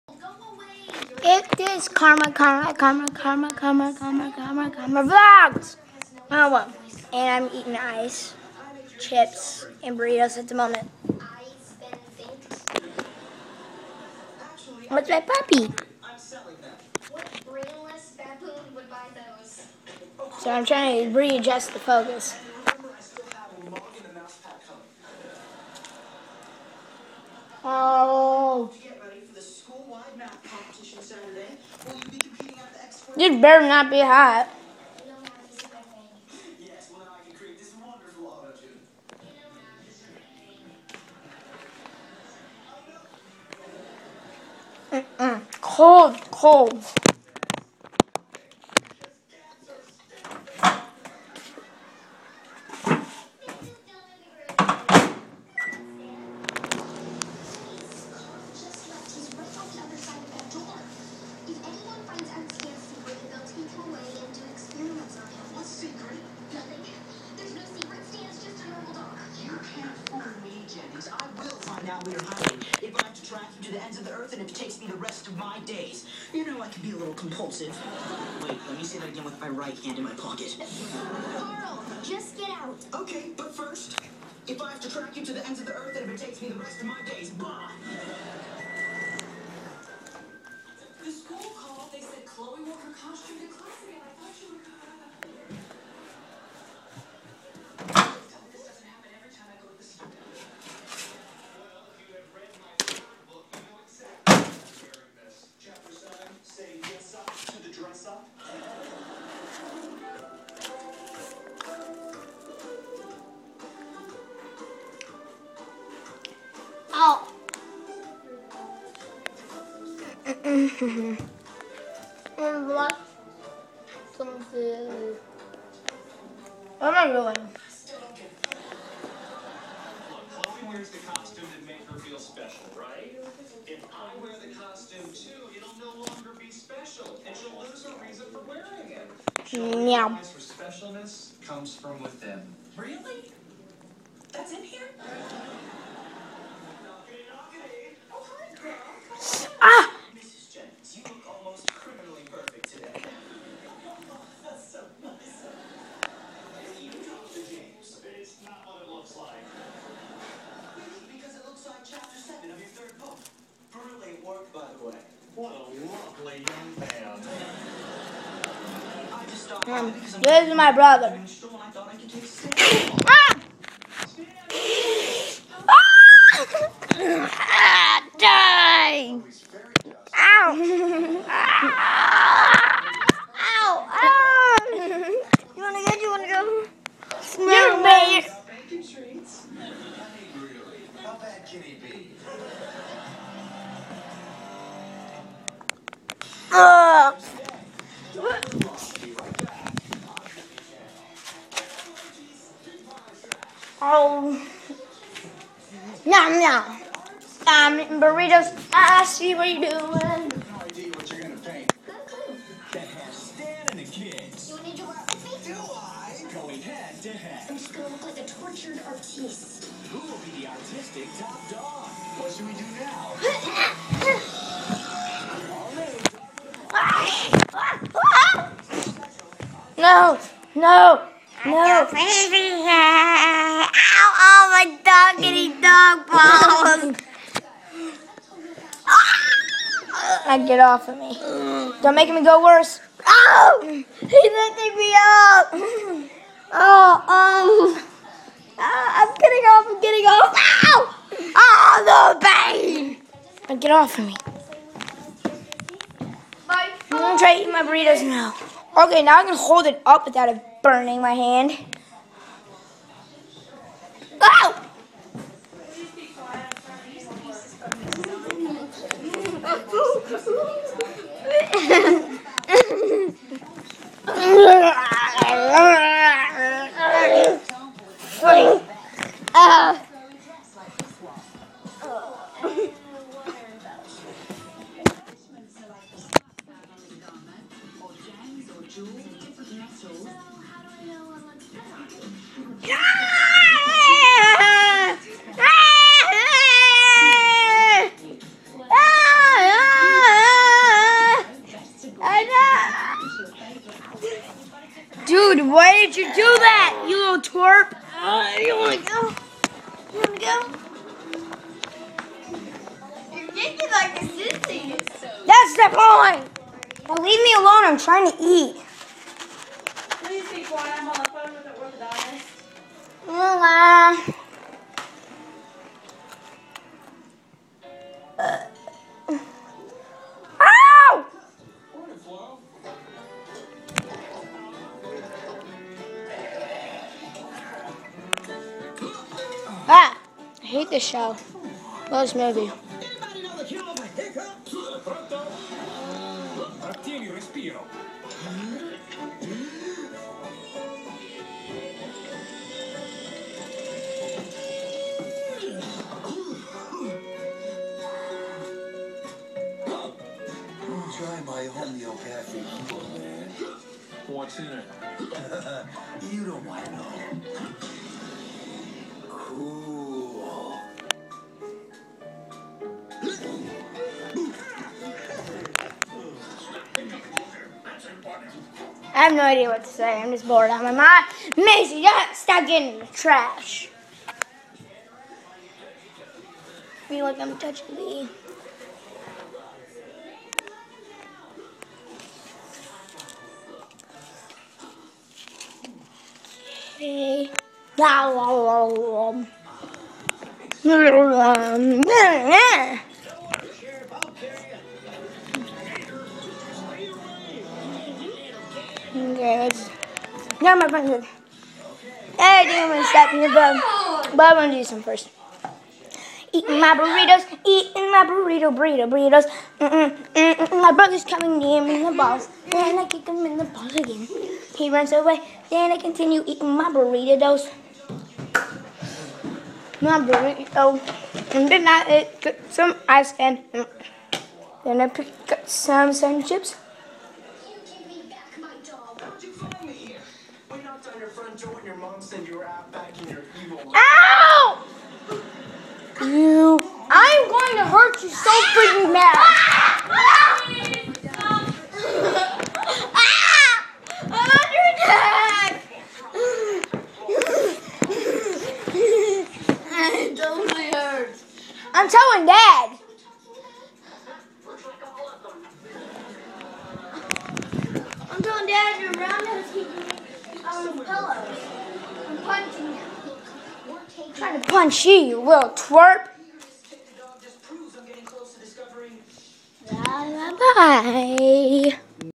Daily Vlogs #1 Burrito Song
Just me eating my burritos and sun chips and ice with my brothers being a twerp